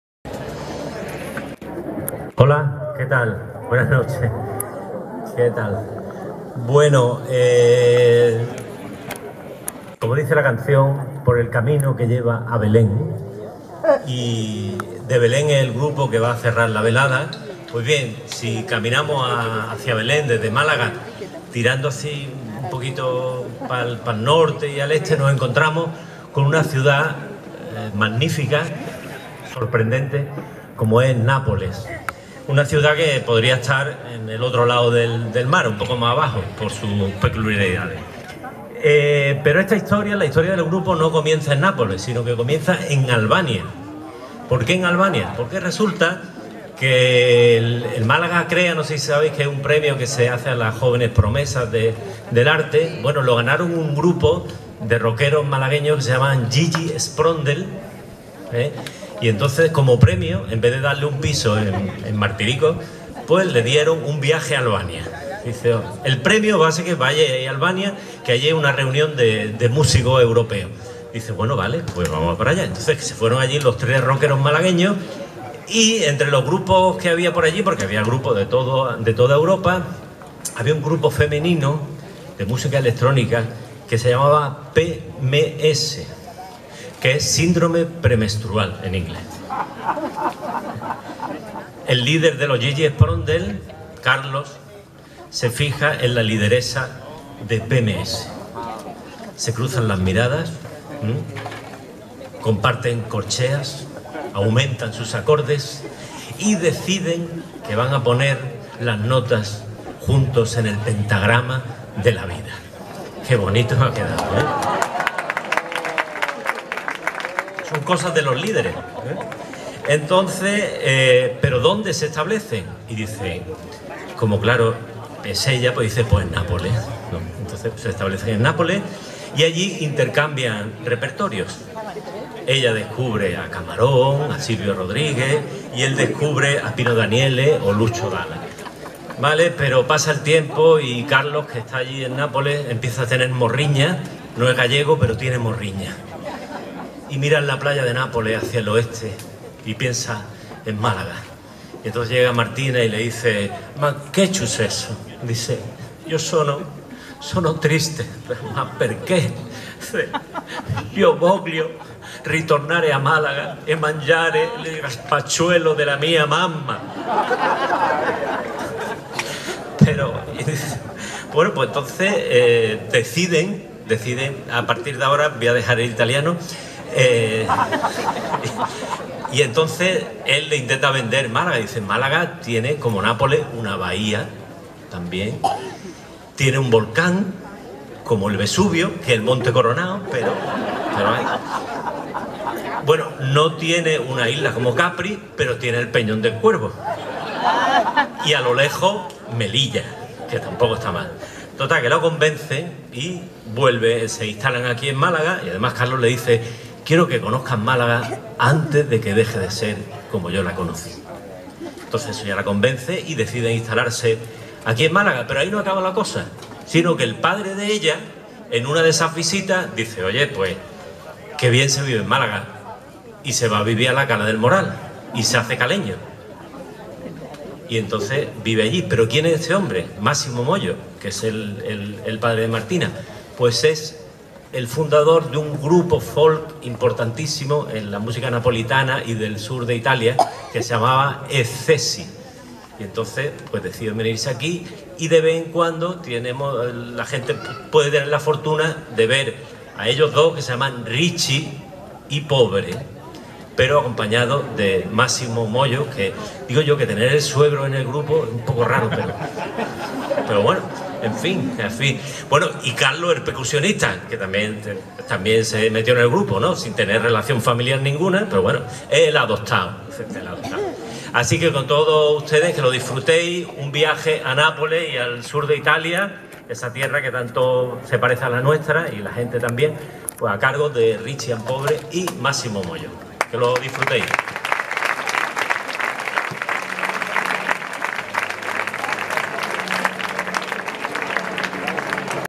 XXI Velada Gibralfaro por Palestina (02) - Eduardo presenta a Ricci & Pobre con Massimo Mollo
Segundo vídeo de la XXI Velada musical de Gibralfaro en solidaridad con Palestina que la Asociación Al Quds Andalucía organizamos el 1/7/2023

El compañero Eduardo presenta al grupo Ricci & Pobre con Massimo Mollo.

¡Siempre puedes colaborar con donativos o asociarte para continuar financiando nuestros proyectos, como el Proyecto Dunya de salud integral de las mujeres palestinas y su "bus rosa".